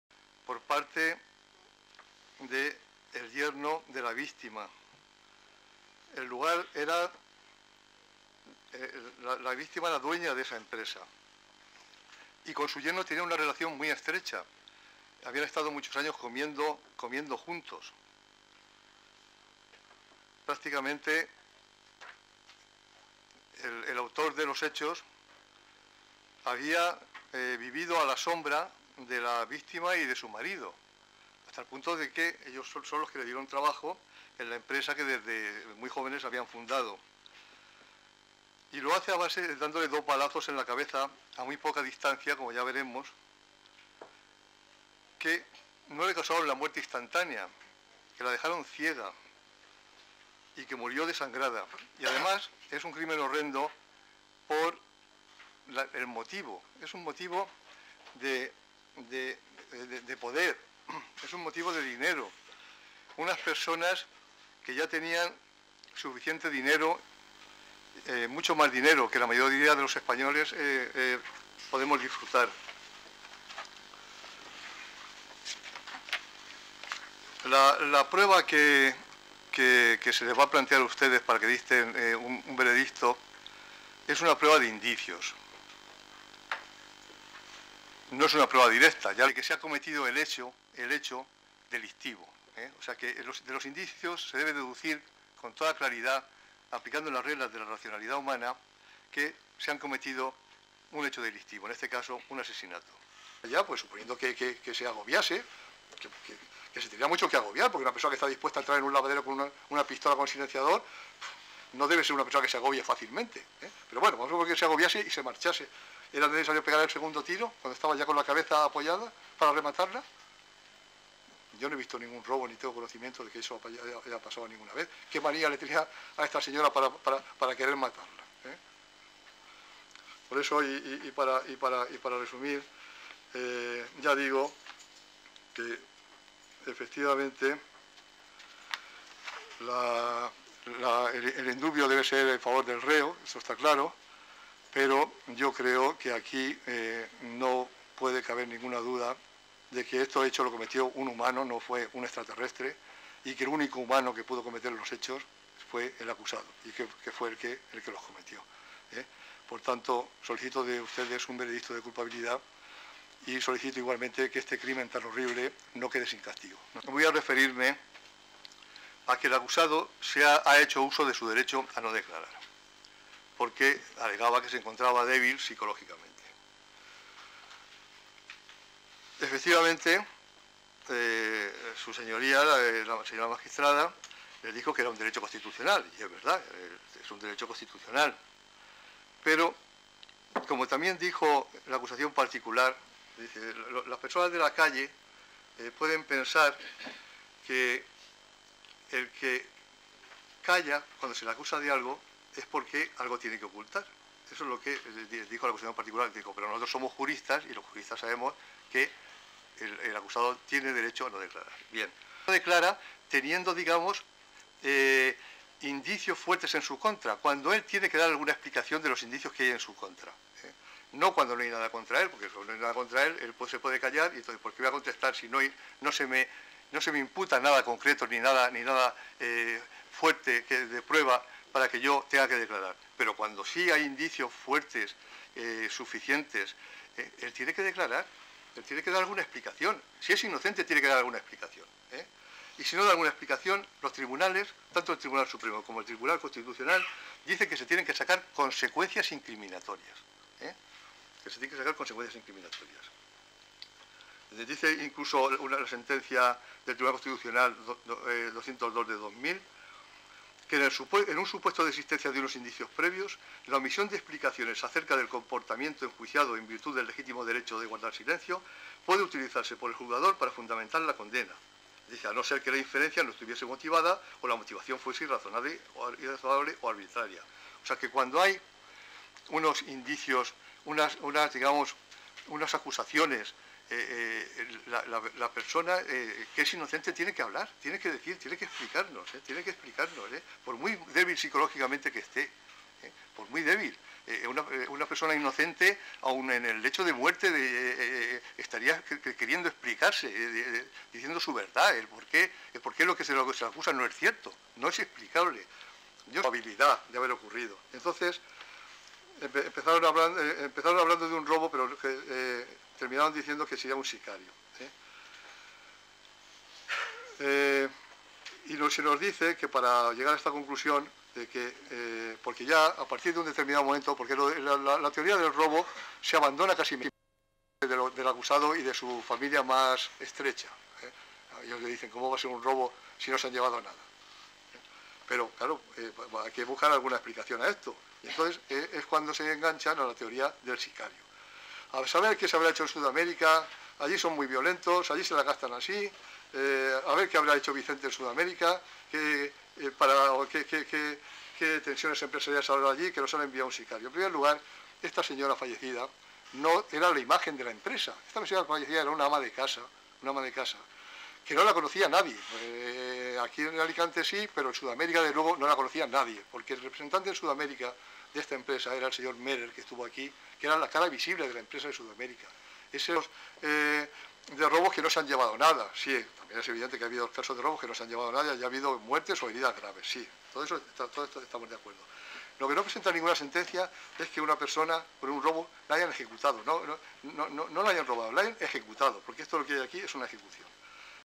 Fiscal pide veredicto de culpabilidad del yerno viuda del expresidente de la CAM
Declaraciones del fiscal, José Llor, en las que ha solicitado al jurado "un veredicto de culpabilidad" para Miguel López, único acusado del asesinato de la viuda del expresidente de la CAM para que "este crimen tan horrible no quede sin condena". Llor ha señalado que el derecho a no declara de Miguel López es porque "algo tiene que ocultar" y ha rechazado que se trate de un robo